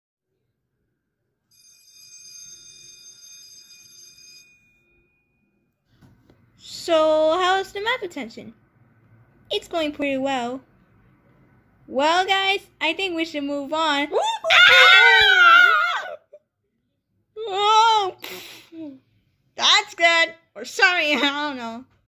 PBS VIDEO | Who Scare Us | The Student Mystery Girl Of School
The school bell ring & it goes very well! but when suddenly, The mystery girl scares p-head and his pals. For a second, Who she might be,
we will figure it out!

Credit to PBS